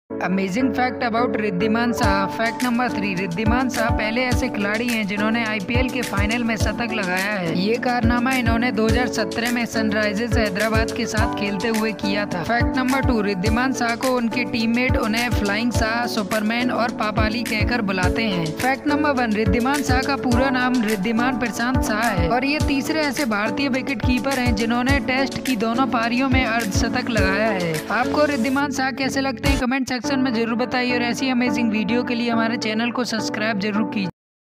Amazing facts about Wriddhiman saha
In this video we are going to be discuss some amazing and interesting facts about Wriddhiman saha 😱.